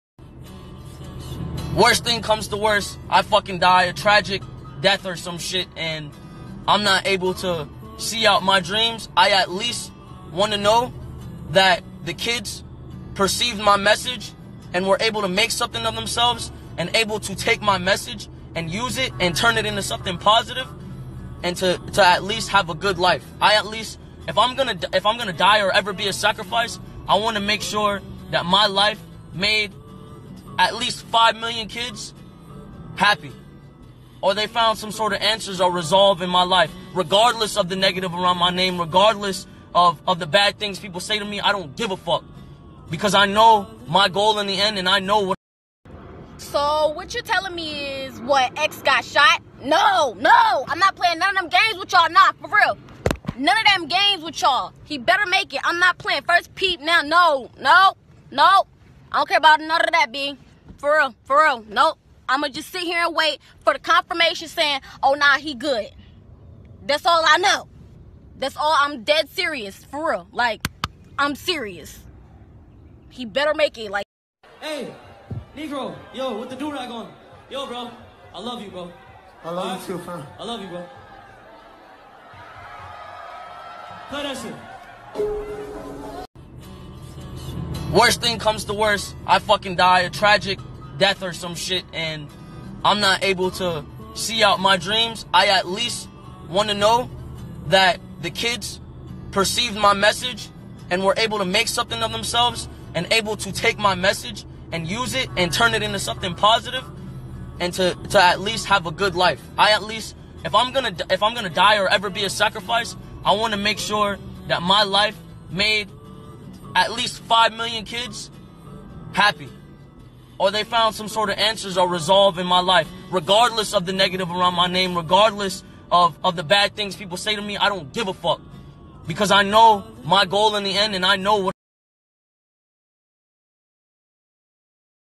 XXXTentacion illuminati Sacrifice 6/18/18**6/18+18=36 THUS 666
DID Rapper XXXTENTACION KNOW HE WAS ABOUT TO BE A SACRIFICED